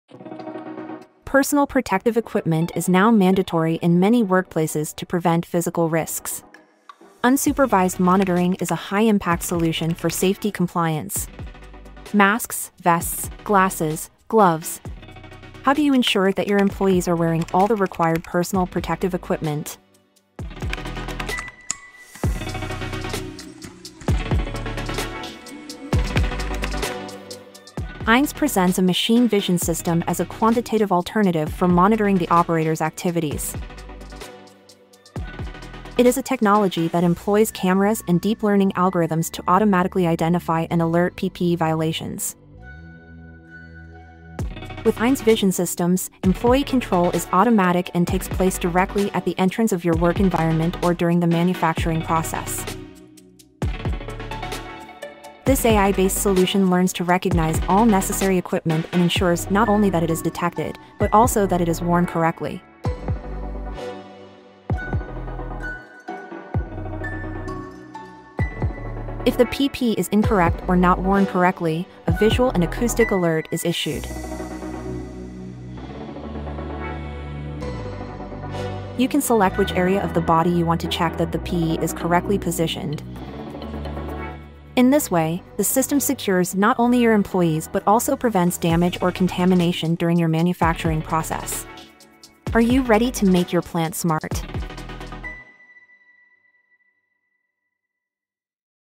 EINES Personal Protective Equipment Detection
PPE is mandatory in many workplaces to prevent physical risks.

How do you ensure that your employees are wearing all the required personal protective equipment?

Eines presents a machine vision system that employs cameras and deep-learning algorithms to automatically identify and alert PPE violations.

This AI-based solution learns to recognize all necessary equipment and ensures not only that it is detected, but also that it is worn correctly.

In this way, the system secures not only your employees but also prevents damage or contamination during your manufacturing process.